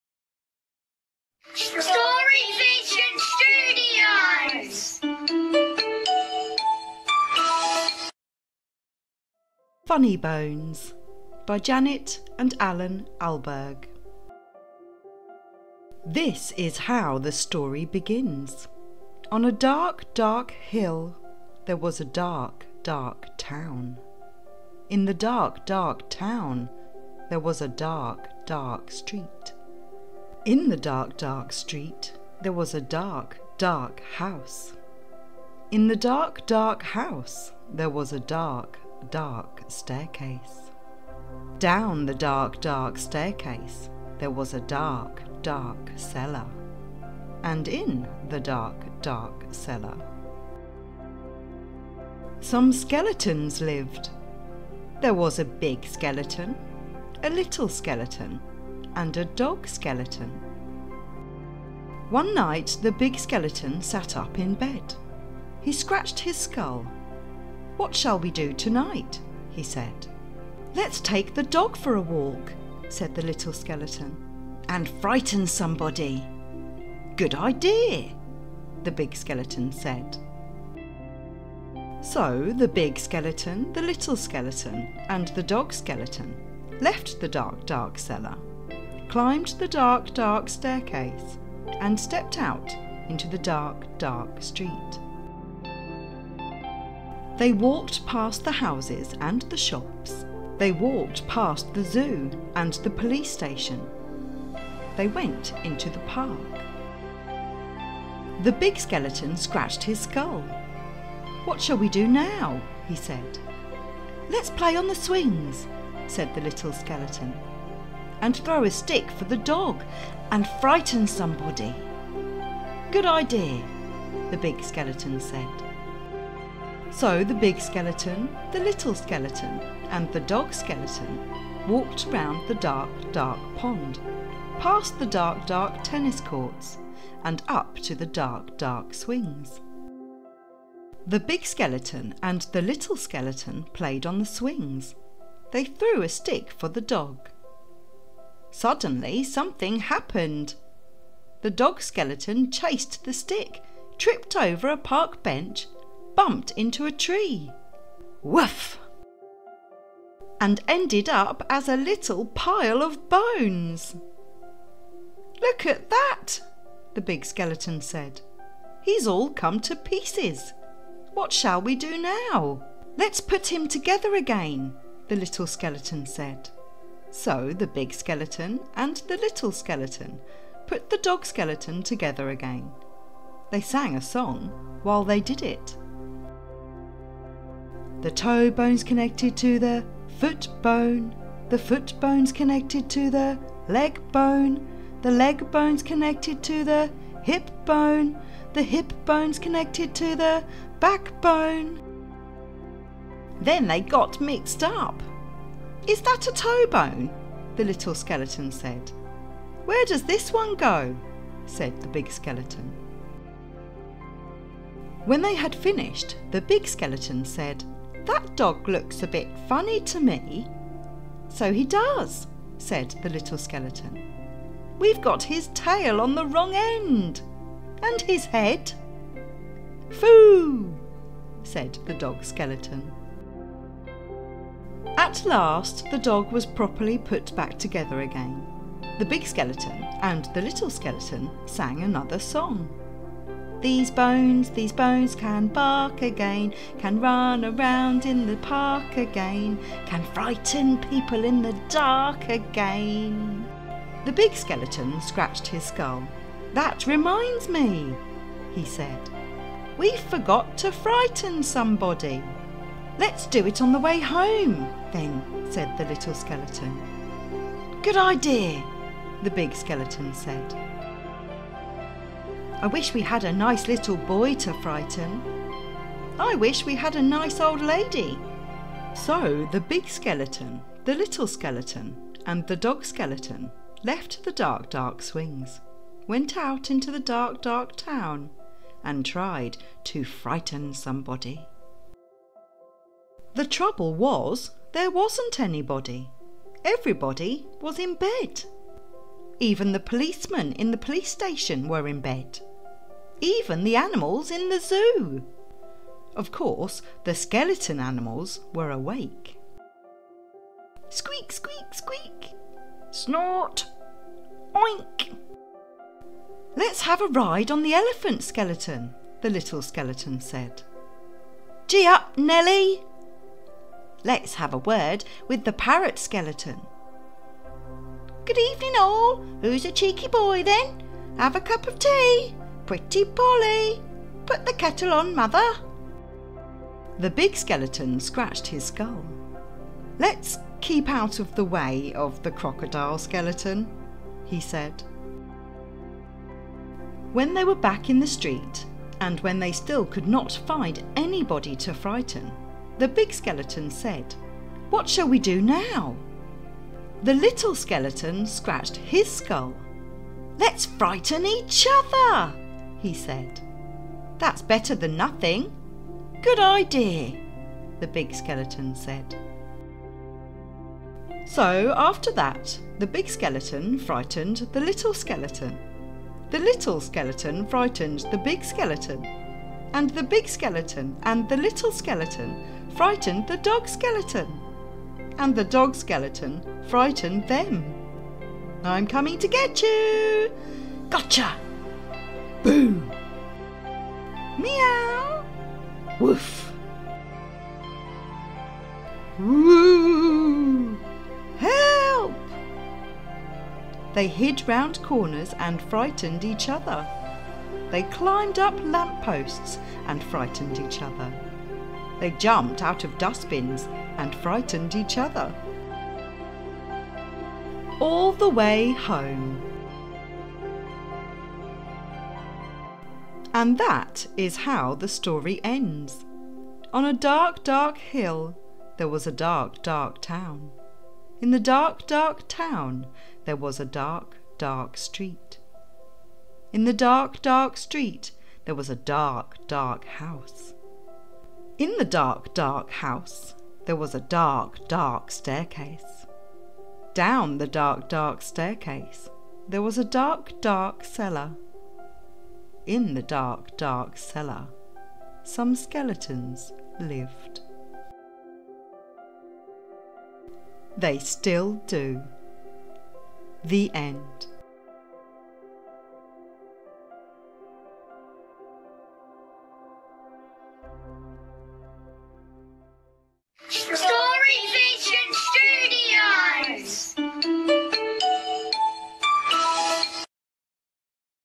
Funny Bones by Janet & Allen Ahlberg. Funny, charming Children's story. Audiobook Read-Aloud.
A charming story about a big skeleton, a little skeleton and a dog skeleton, who live in a cellar. Bored one night, they set off for a dog walk and to frighten somebody, but nobody is around, everyone's asleep! They manage to have fun anyway!

Musician: @iksonmusic